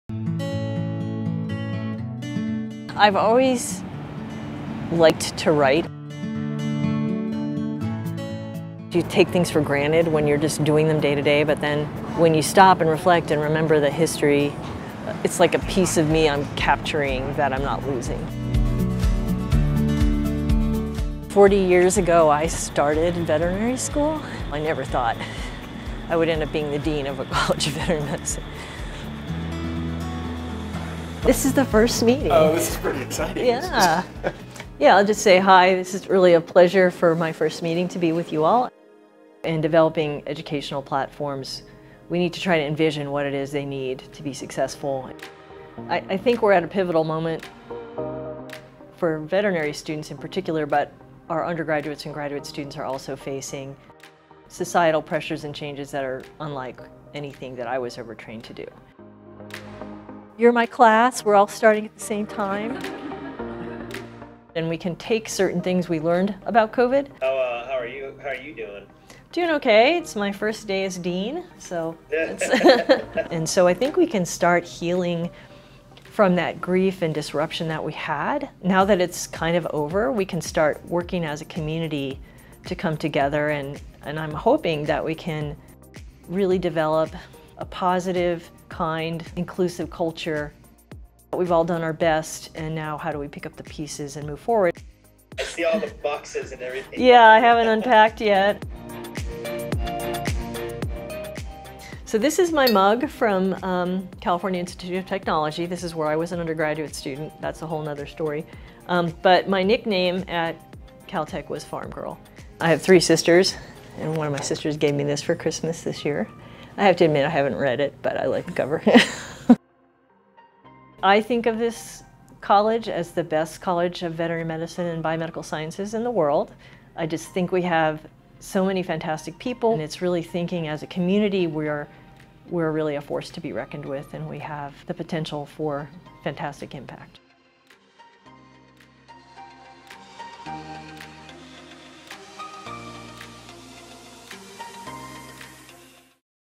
From "farm girl" to dean
Dr. Sue VandeWoude became dean of the CSU College of Veterinary Medicine and Biomedical Sciences on Aug. 16, 2022. This video follows her first couple of days as she honors her past and looks ahead to the future of education in biomedical sciences and veterinary medicine. Videographer: Dane Aragon, Translational Medicine Institute